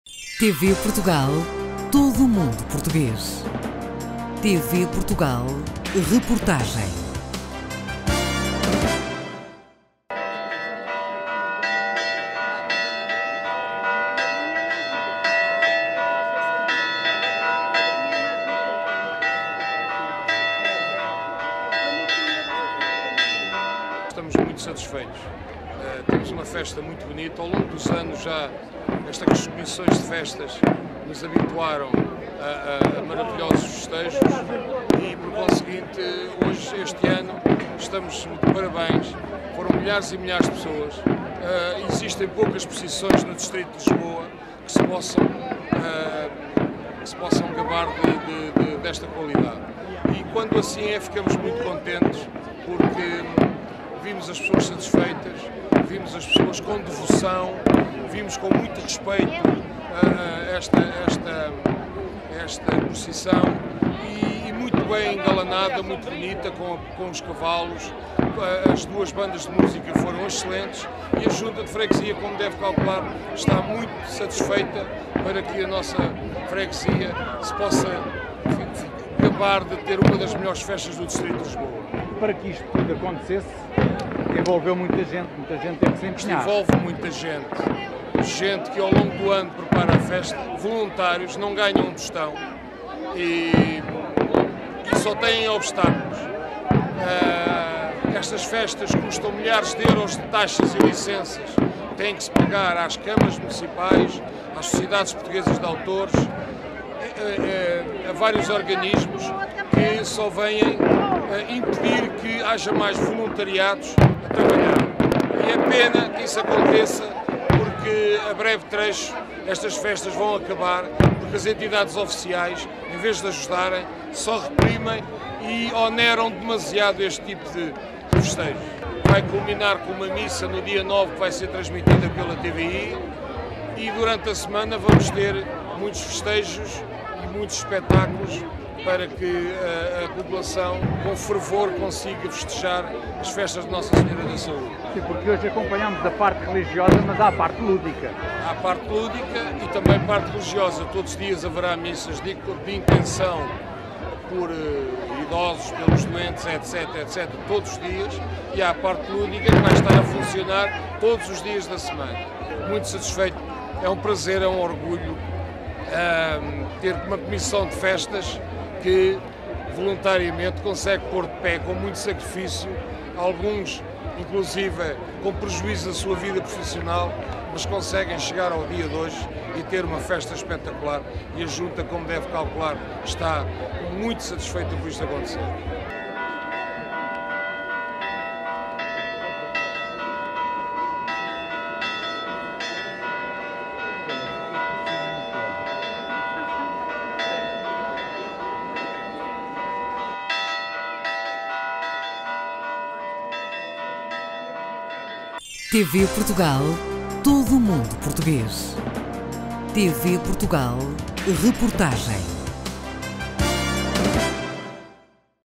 Nossa Senhora da Saúde
Guilherme Ponce de Leão, Presidente da Junta de Freguesia de São João das Lampas, feliz pela participação de residentes e furasteiras na Procissão da Nossa Senhora da Saúde. Contou com a participação de Marco Almeida, Vice Presidente da Câmara Municipal de Sintra. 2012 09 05